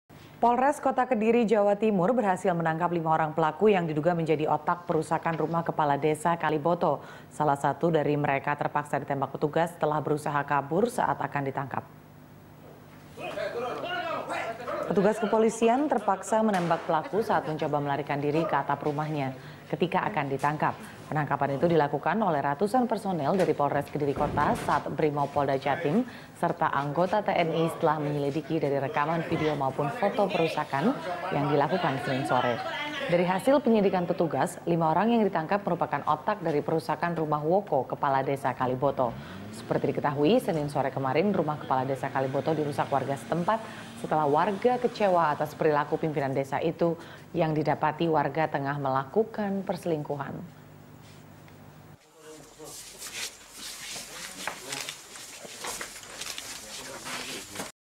5 Perusak Rumah Kades Kaliboto Diringkus
Jajaran Kepolisian Resor Kota Kediri, Jawa Timur, menangkap 5 pelaku yang diduga menjadi otak perusakan rumah kepala desa Kaliboto. Seorang dari mereka ditembak petugas setelah berusaha kabur ke atap rumah.

Facebook.com/BeritaSatuTV
Youtube.com/BeritaSatu